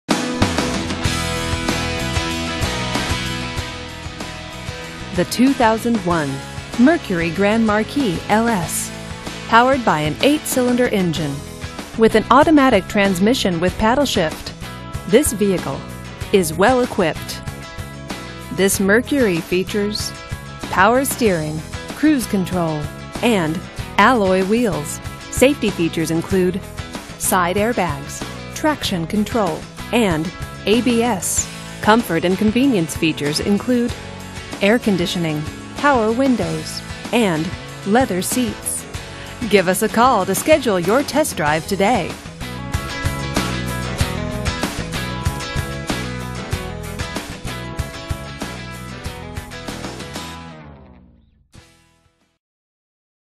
2001 Mercury Grand Marquis LS - Harry's Quality Cars, Reno
Harry's Quality Cars

2001 Mercury Grand Marquis LS

$6,999

Color: Gold
Engine: 8 Cyl.
Transmission: Automatic With Overdrive
Miles: 82018
VIN: 2MEFM75W51X623152
Stock: 10992P

This vehicle features: RWD, Cruise Control, Power Adjustable Pedals, Power Steering, Power Brakes, Power Door Locks, Power Windows, Radial Tires, Alloy Wheels, Air Conditioning, Tilt Steering Wheel, Rear Defroster, Remote Trunk Lid, Remote Fuel Door, Accent Stripes, Body Side Moldings, Power Passenger Seat, Climate Control, Driver Side Remote Mirror, Compass, Anti-Lock Braking System, AM/FM Stereo Radio, Security System, Woodgrain Dash, Center Arm Rest, Power Driver's Seat, Traction Control, Leather Upholstery, Cassette Player, Passenger's Front Airbag, Driver Side Air Bag, Split Front Bench Seat, Keyless Entry, Homelink System, Lumbar Seat

[DFIN:2:2283148:A:78859D1D:1b8ea5239cac63431490e821c3b87c84]